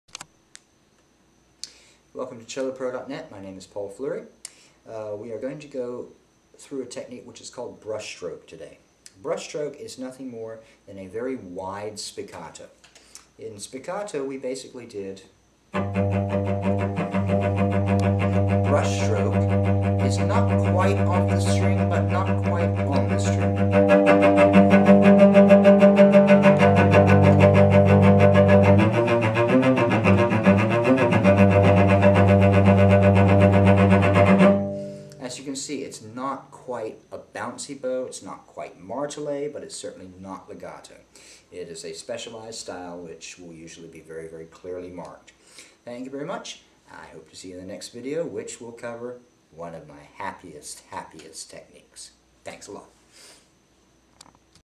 Brush Stroke for Cello Explained (Tutorial)
Correct technique for a brush stroke for cello.